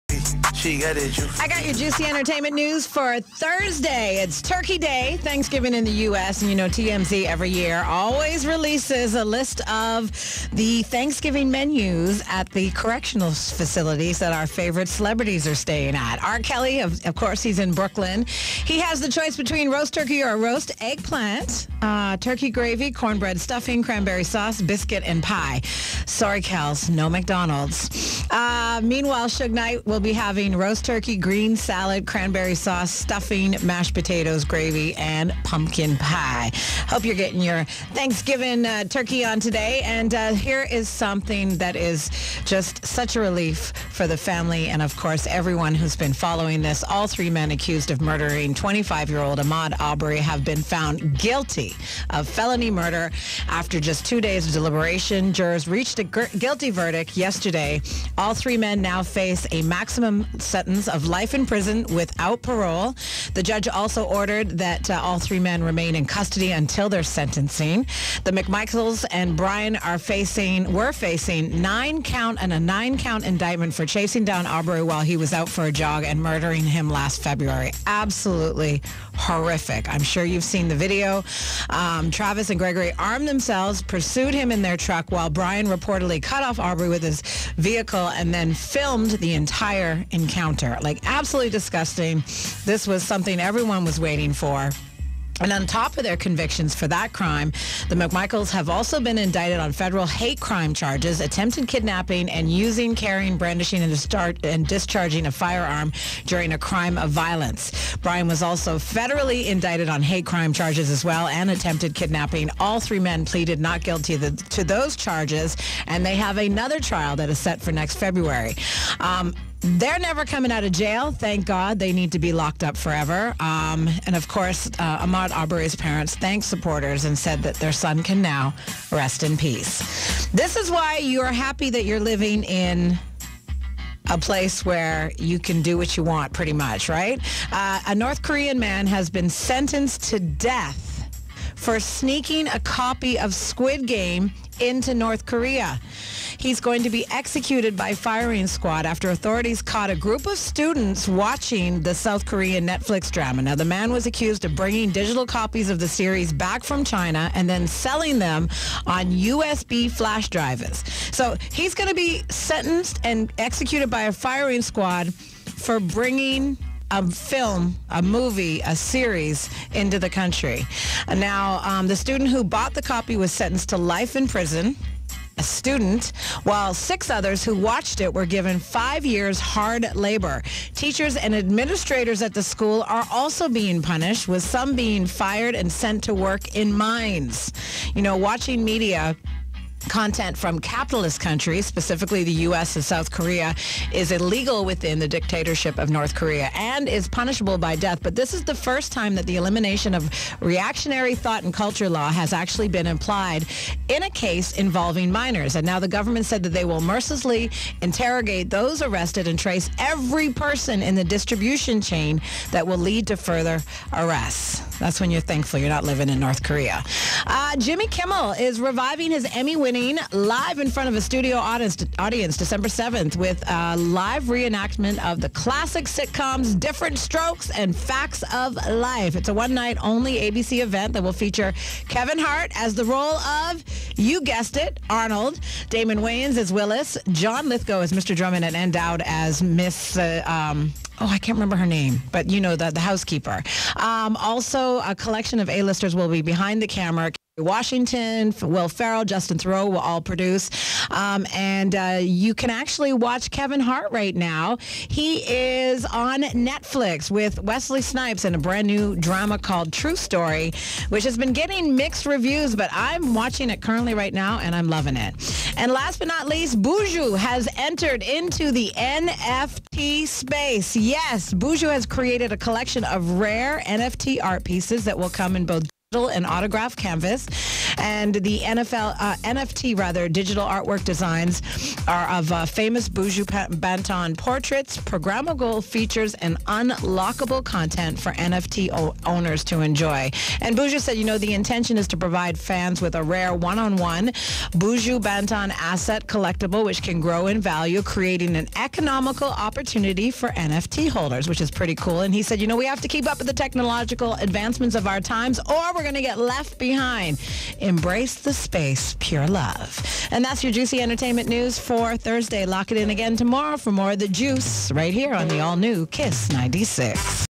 North Korean sentenced to death For watching Squid Game
The Juice With JJ - North Korean sentenced to death For watching Squid Game